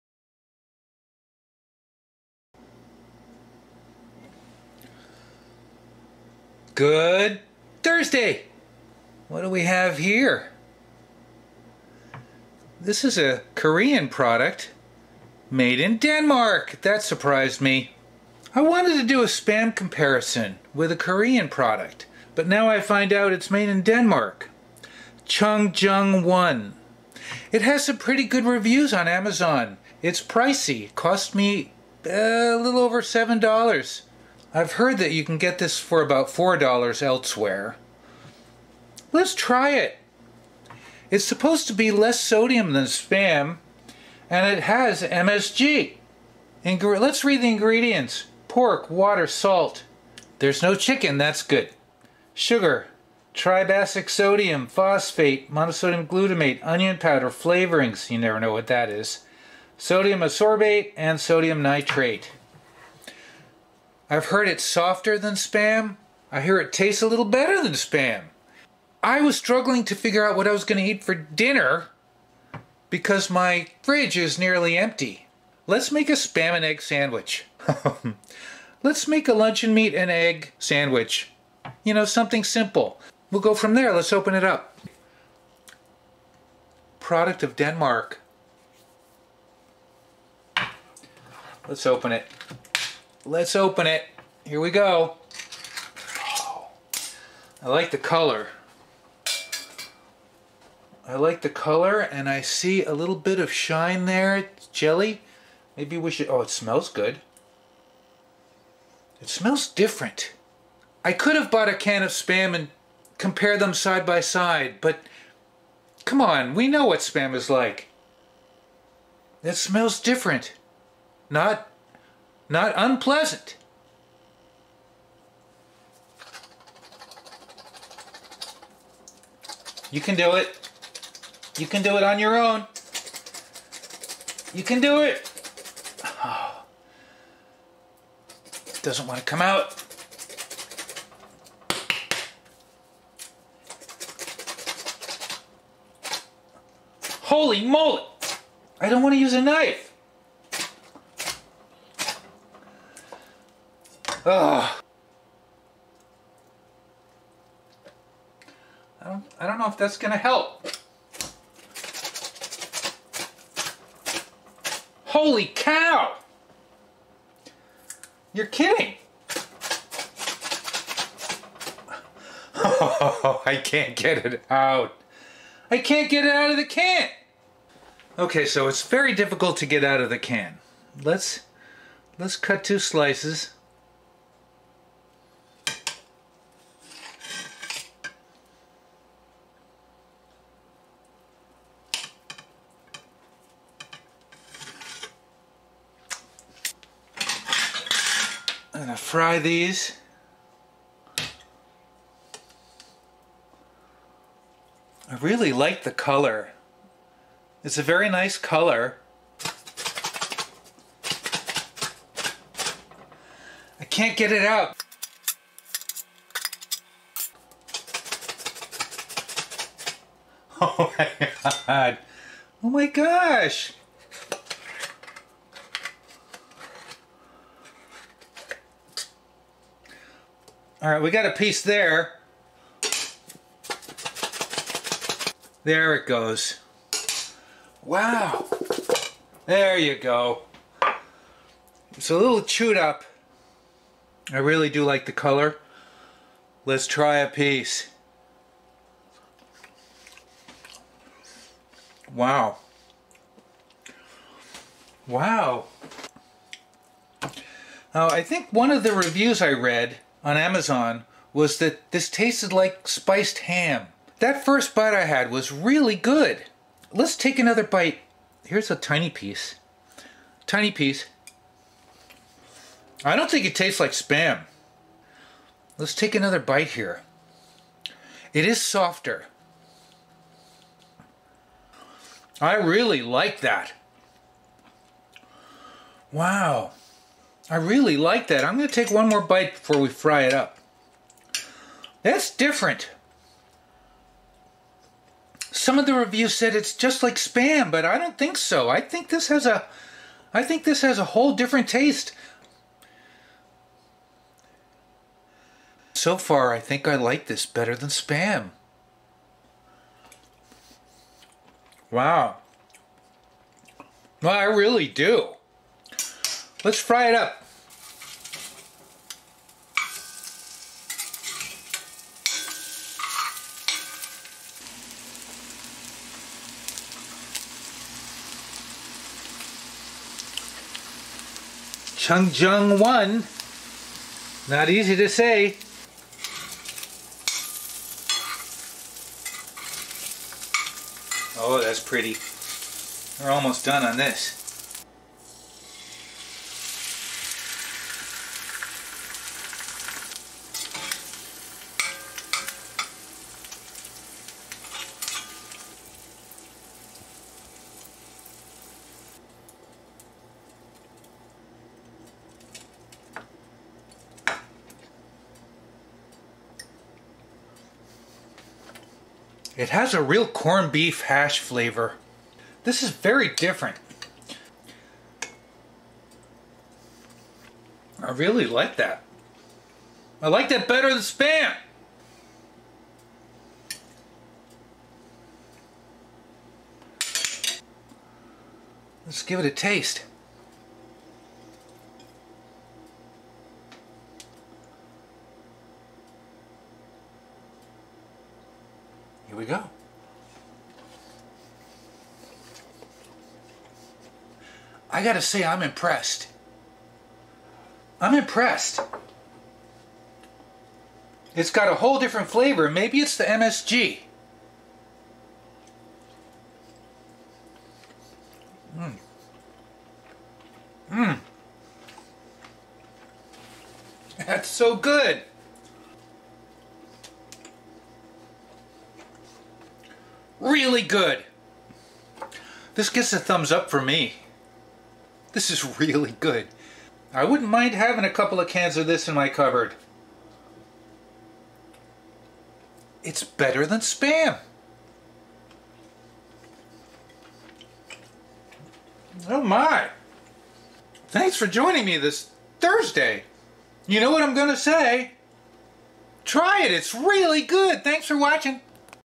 Chung Jung One Canned Luncheon Meat Review. Is It Better Than SPAM?
Trying Korean Chung Jung One Canned Luncheon Meat. Is it better than SPAM? Can I get it out of the can? Sharing : )